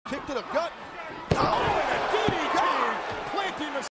DDT
Eddie Guerrero spikes Rey Mysterio with a DDT.